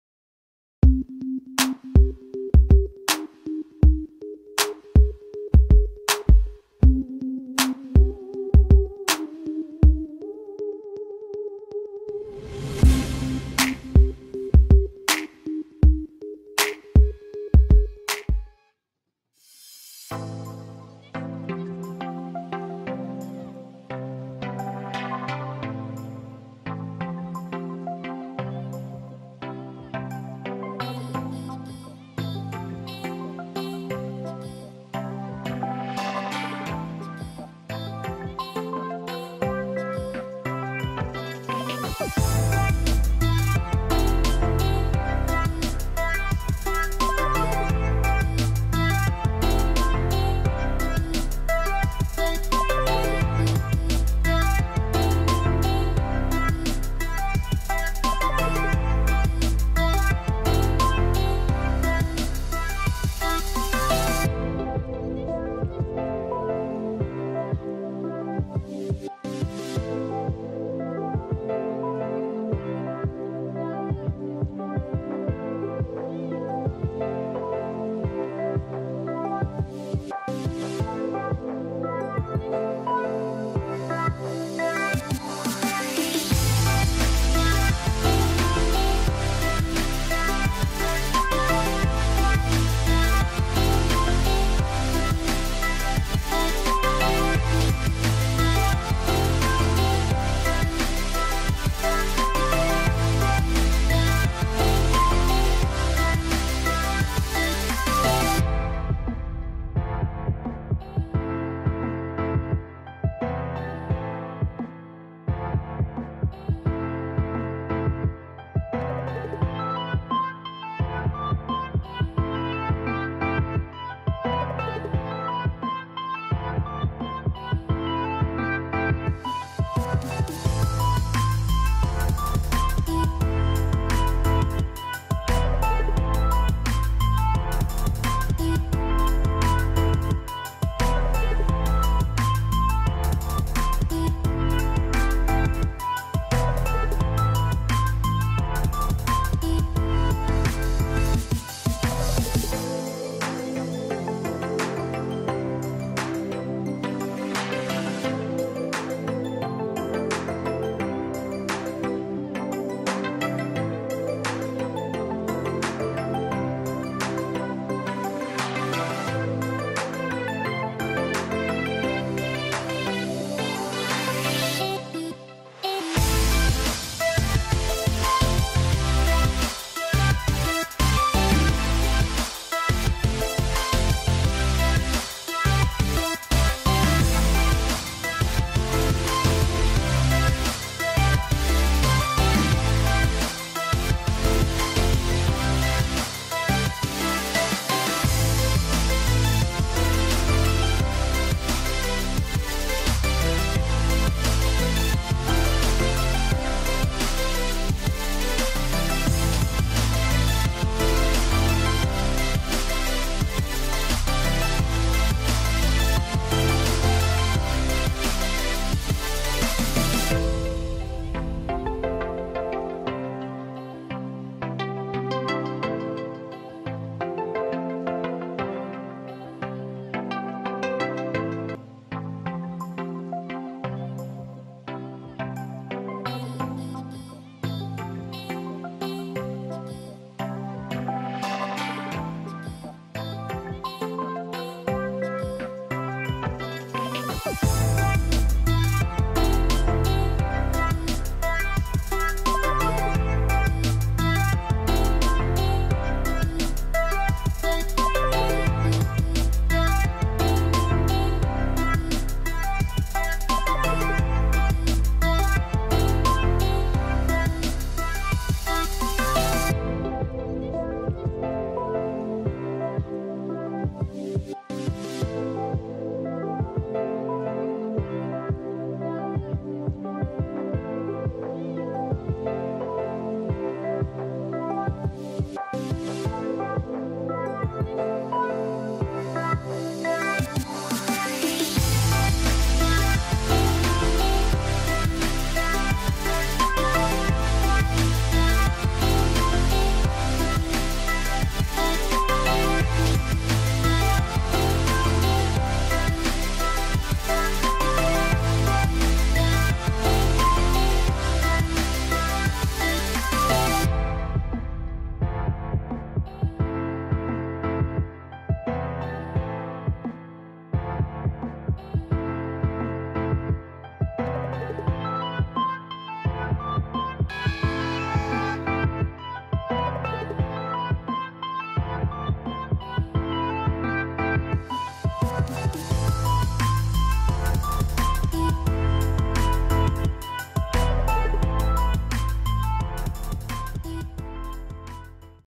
Bokor Mountain | A Cloudy Heaven - Kampot, Cambodia
One of the best tourist attractions in Kampot, Cambodia, you should come one in your lifetime to see its natural beauty, french colonial architecture, Khmer pagoda, and many more. Bokor Mountain is most suitable for a family picnic, and a group road trip.

To access the location, here you go:
1- Rainbow Roundabout: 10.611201, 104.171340